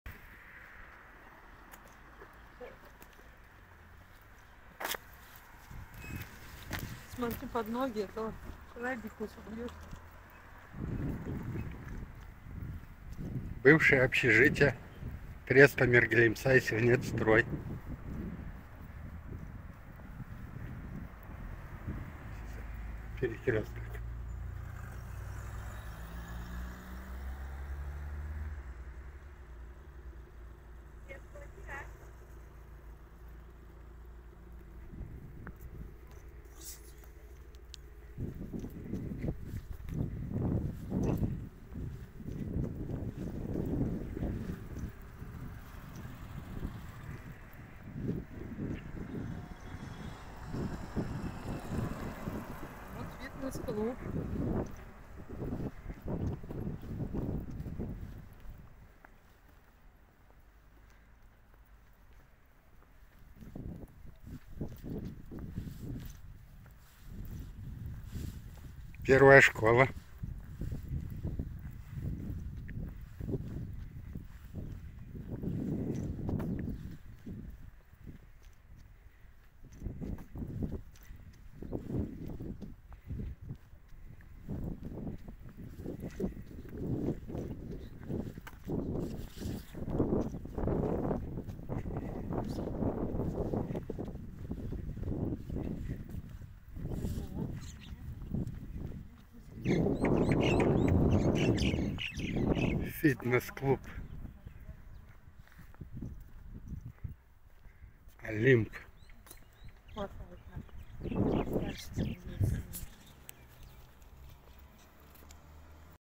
Кентау. Новая дорога от ул.Аль-Фараби до проспекта А.Яссави. 29 октября 2023. Туркестанская область
rinkar kentau кентау Кентау. Новая дорога от ул.Аль-Фараби (Добролюбова)до проспекта А.Яссави (50 Лет Октября). 29 октября 2023 года. Туркестанская область Казахстан.